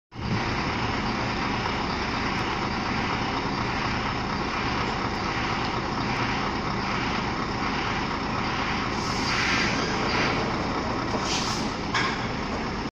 Ainscough Crane Hire
Ainscough Crane Hire Scania ballest truck seen on Leith street Edinburgh